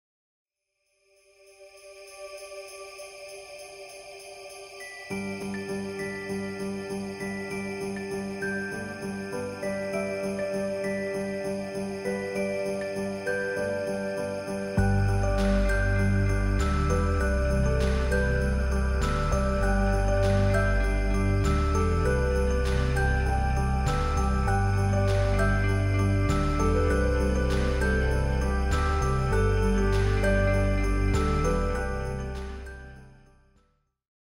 Dona Gelsinger Snowfall Angel Fiber Optic Lighted Christmas Canvas Wall Art
The Snowfall Angel brings winter finery to grace your home this holiday season. She carries a garland of red poinsettias and green pine. Fiber optics light the garland greenery and twinkle merrily in the background, resembling falling snow.